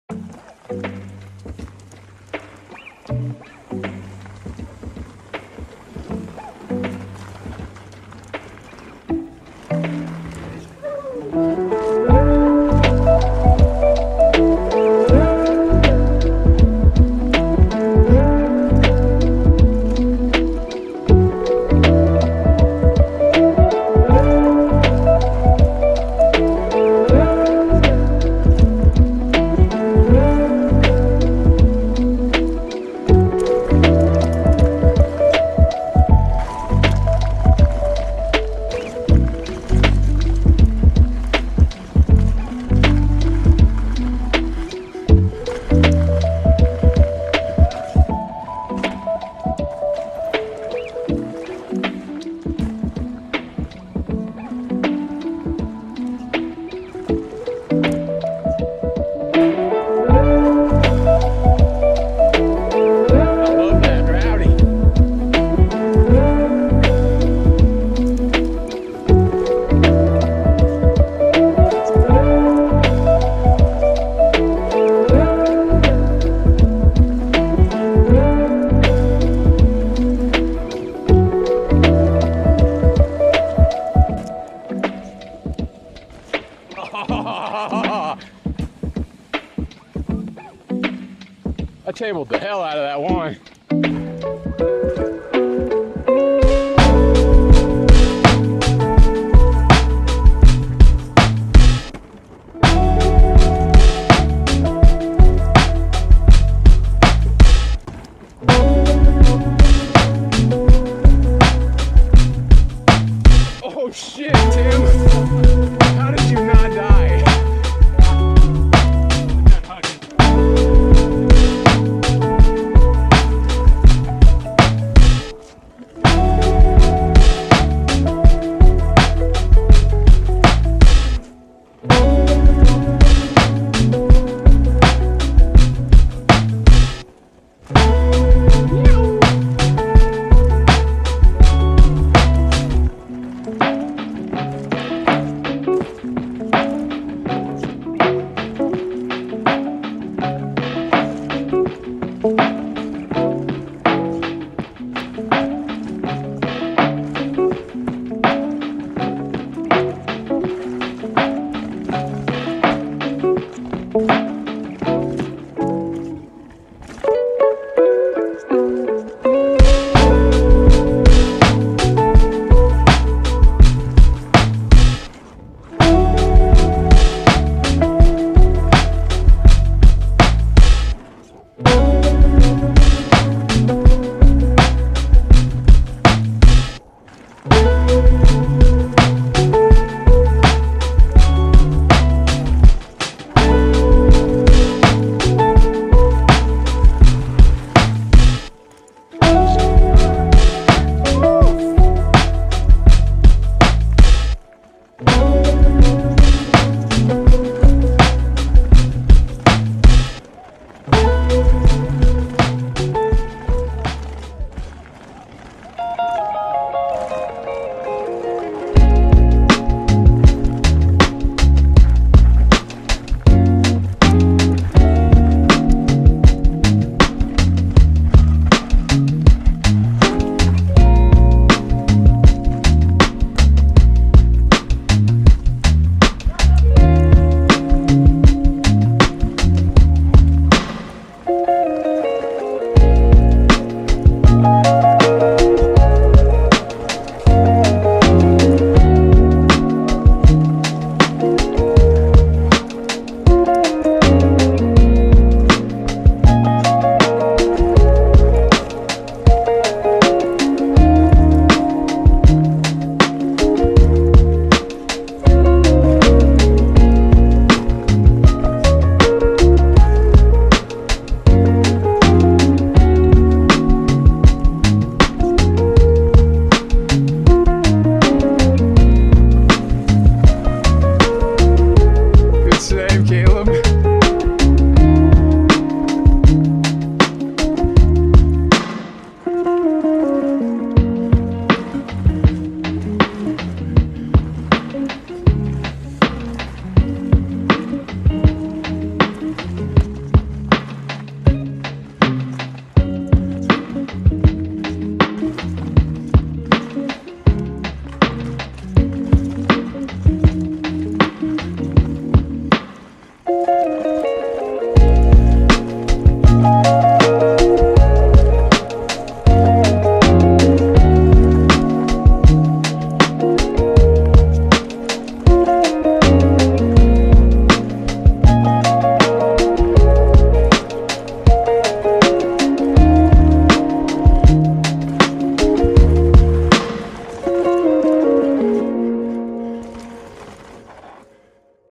TACO CAT SCRAMBLE AT HOWLER BIKE PARK
Finally getting around to editing the footage from a homies trip to @howlerbikepark. I hit the Taco Cat B line and scrambled it a few times with the rest of the riding homies: @smalltimenobody @timkaeser7363 Grant and Caleb.

Bike: 2022 YT Capra Core 3 MX 🟨